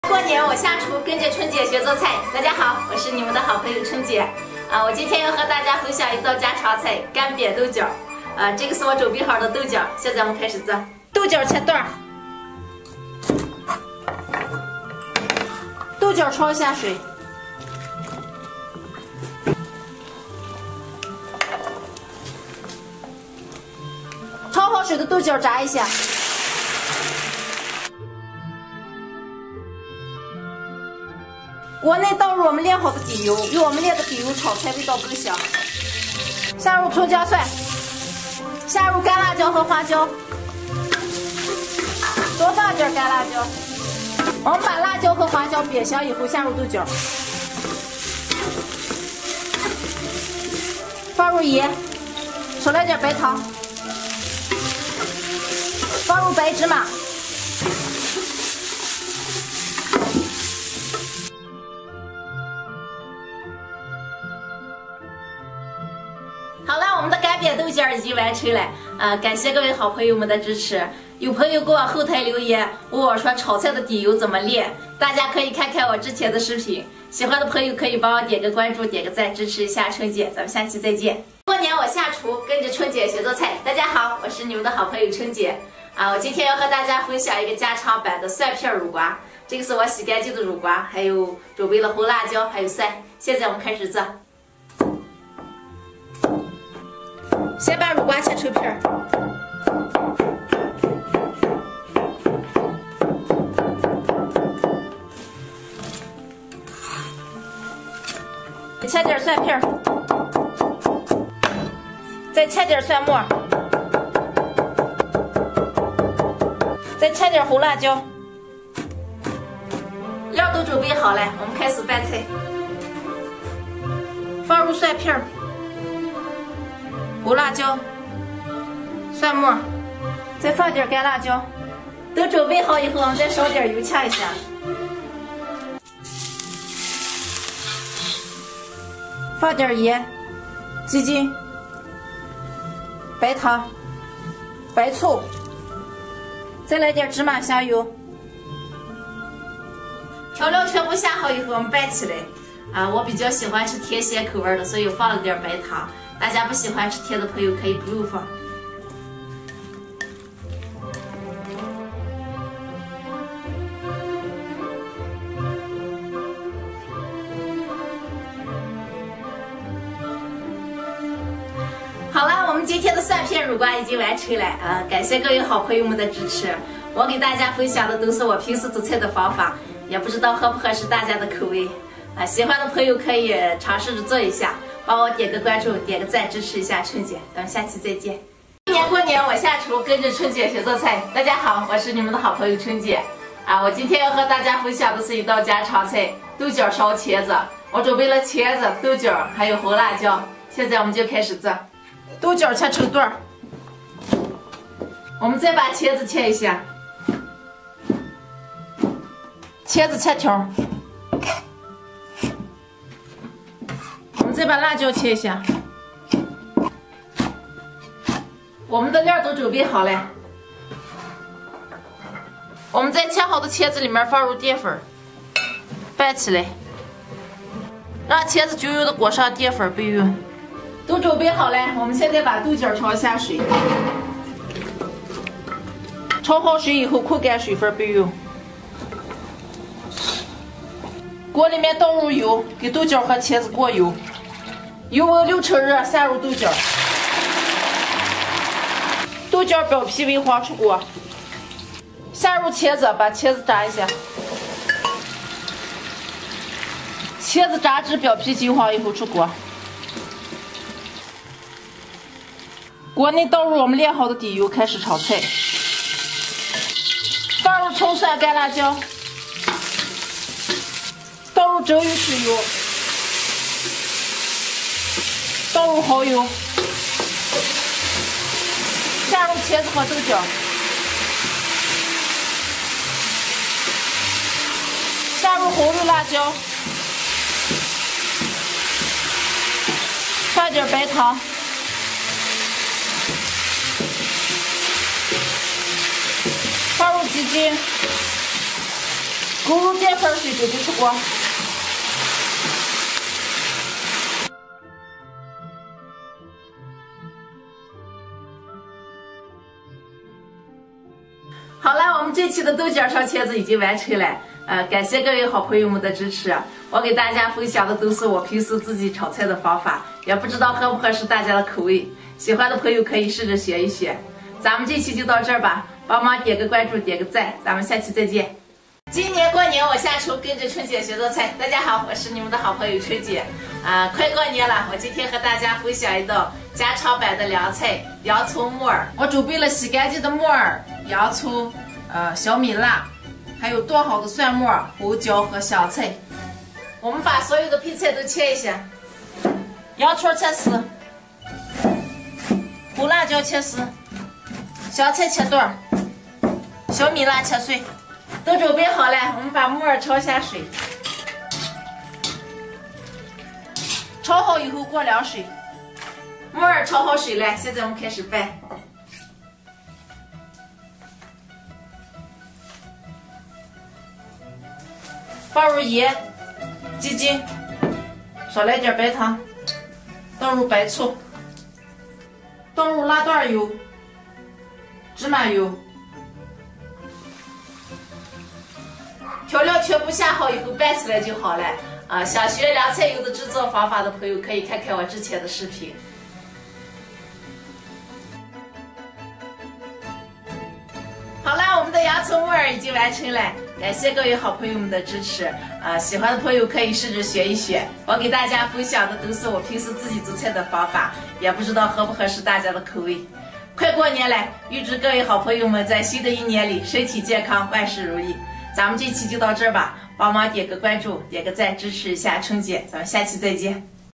家常拿手菜干煸豆角，学会这个技巧，香辣爽口超下饭，给肉都不换！【丹霞春姐】
"在视频制作中，我们特别关注了食材的获取，以确保它们不涉及任何形式的动物虐待。"
跟着春姐学做菜，天天都有新菜品，今天做素菜干煸豆角

大家好，我是春姐，